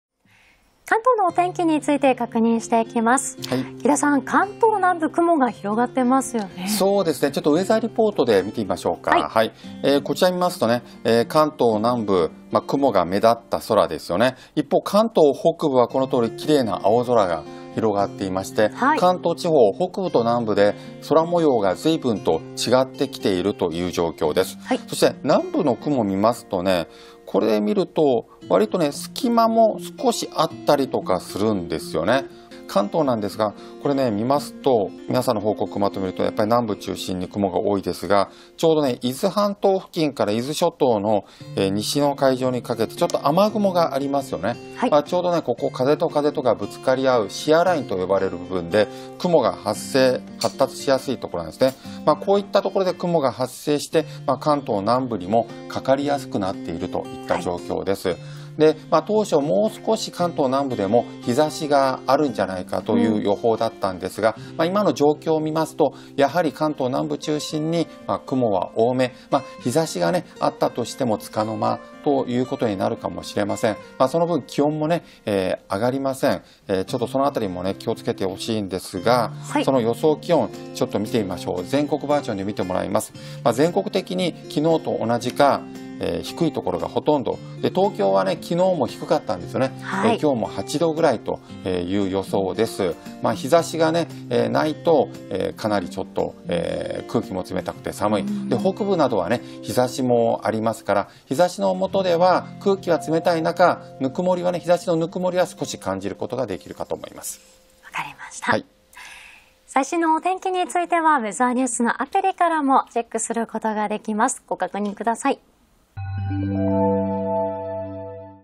【見解変化】関東南部は「日差し多め」→「雲多め」へ
今日1月17日(火)は、関東から西日本にかけての太平洋側は日差しの届くところが多くなっています。ただ、関東南部は雲が多く残っており、見解の変化が出てきました。今後の状況を解説していきます。